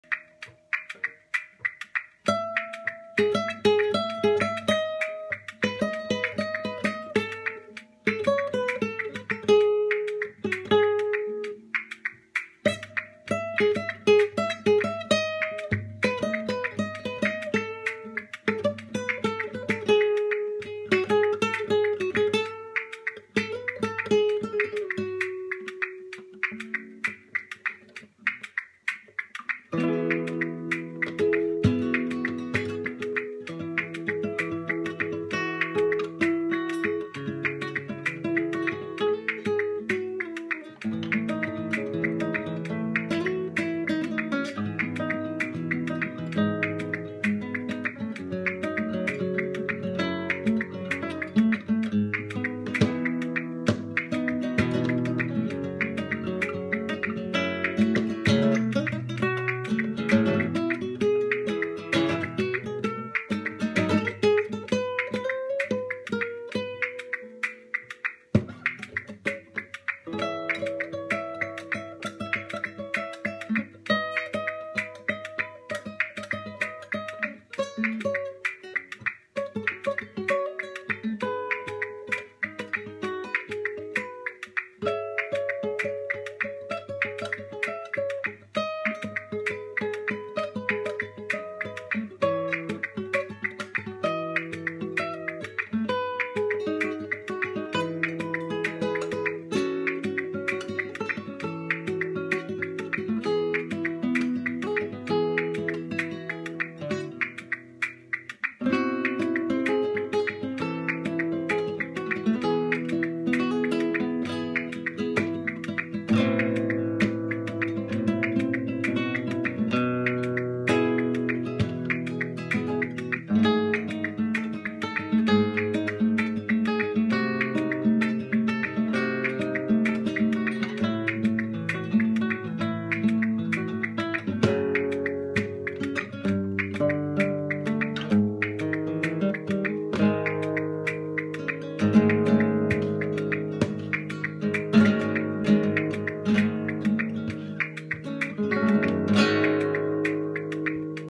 Feb 2016 composition -- Bryan Douglas
A sketch for a new composition... This is a short part taken from somewhere in the middle of the song.